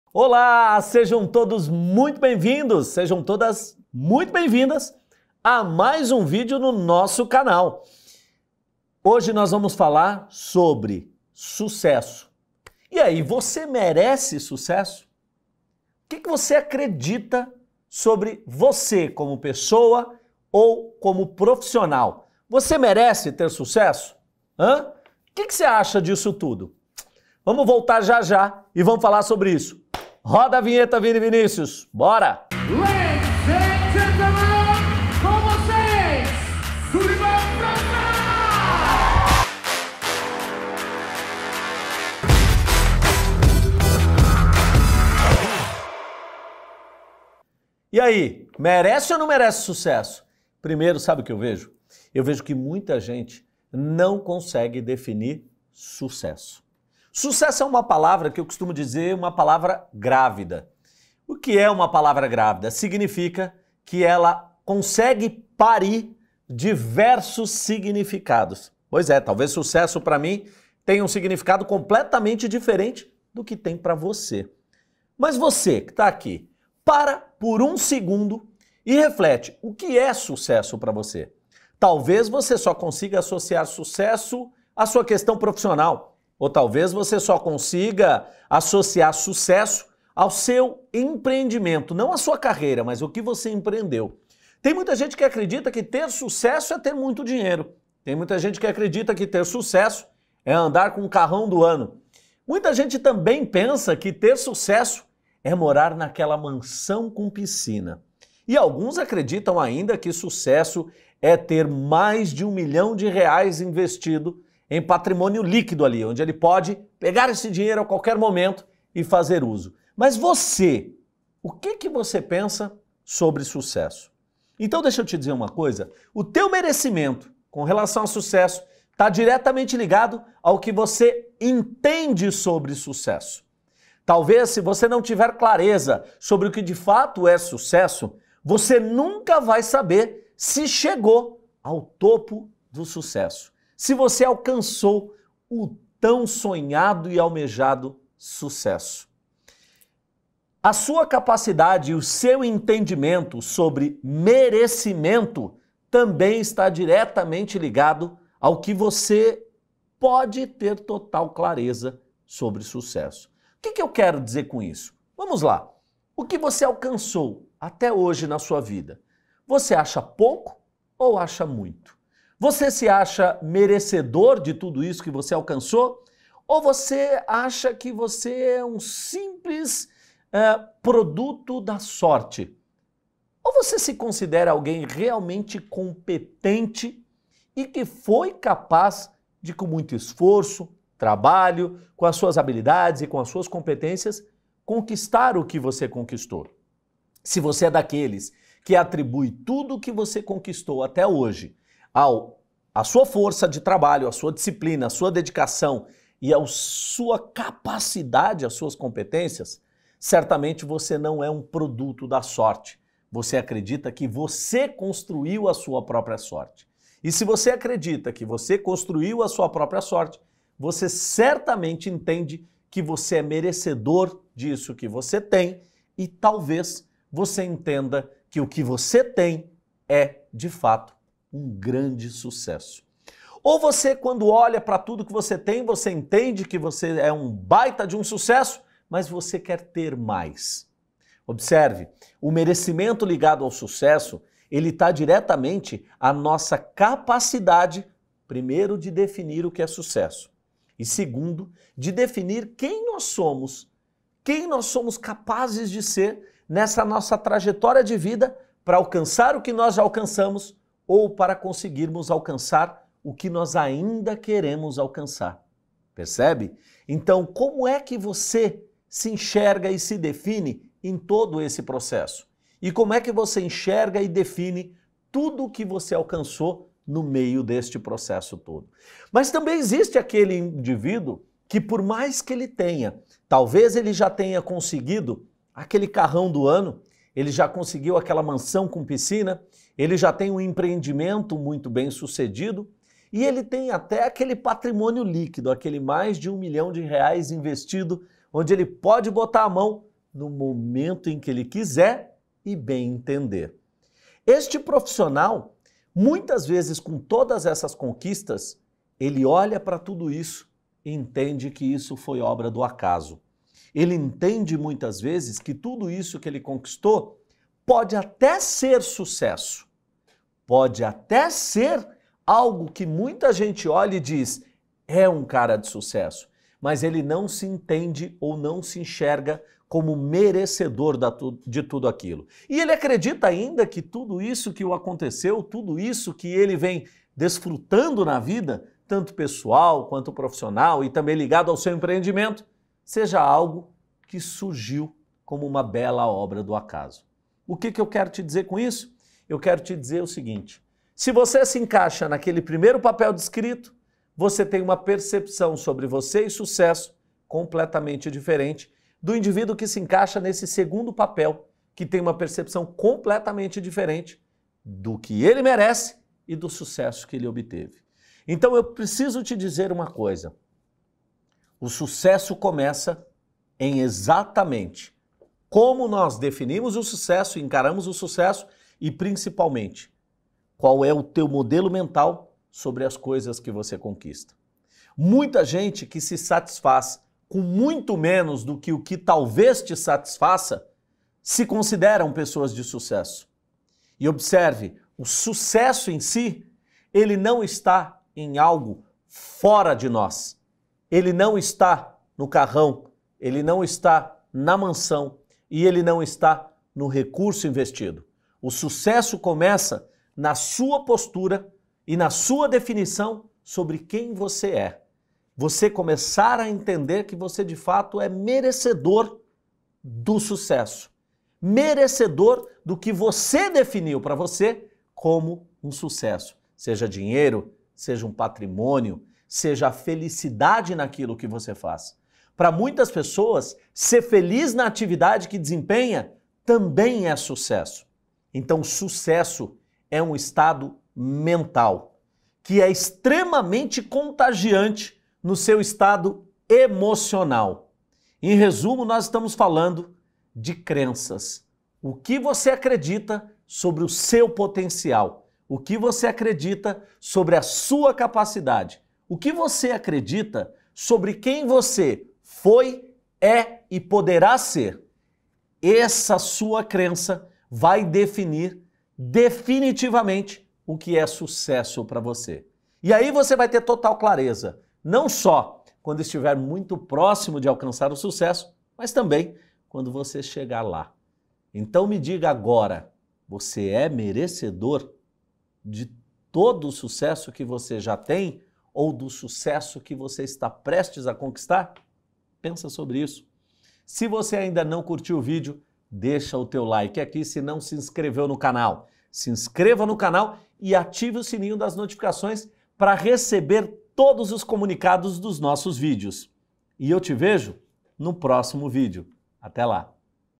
VOCÊ MERECE TER SUCESSO? | Sulivan França
Vamos falar sobre sucesso? O que você acredita sobre você, como pessoa ou como profissional? Você é merecedor de sucesso? Confira!